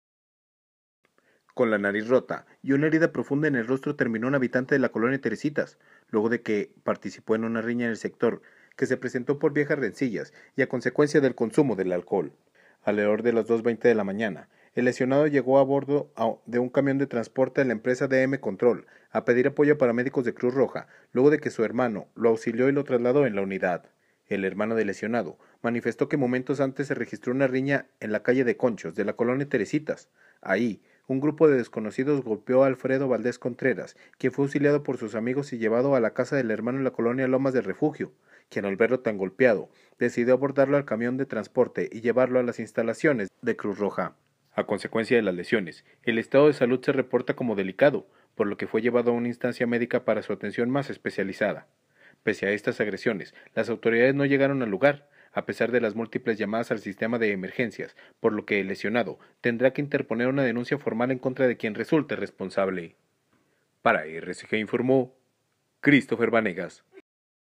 Golpeado llega a pedir apoyo a Cruz Roja
Con la nariz rota y una herida profunda en el rostro terminó un habitante de la colonia Teresitas, luego de participar en una riña del sector, que se presentó por viejas rencillas y a consecuencia del consumo de alcohol.

Alrededor de las 2:20 horas el lesionado llegó a bordo de un camión de transporte de personal de la empresa DM Control a pedir apoyo a paramédicos de Cruz Roja, luego de que su hermano lo auxiliara y trasladará en la unidad.

 El hermano del lesionado manifestó que momentos antes se registró una riña en la calle Conchos, de la colonia Teresitas; ahí  un grupo de desconocidos golpeó a Alfredo Valadez Contreras, quien fue auxiliado por sus amigos y llevado a casa de su hermano en la colonia Lomas del Refugio, quien al verlo tan golpeado decide abordarlo al camión de transporte y llevarlo a Cruz Roja.

A consecuencia de las lesiones el estado de salud se reporta como delicado, por lo que fue llevado a una instancia médica para una atención más especializada.

 Pese a esta agresión las autoridades no llegaron al lugar, a pesar de las múltiples llamadas al Sistema de Emergencias, por lo que el lesionado tendrá que interponer una denuncia formal en contra de quien resulte responsable.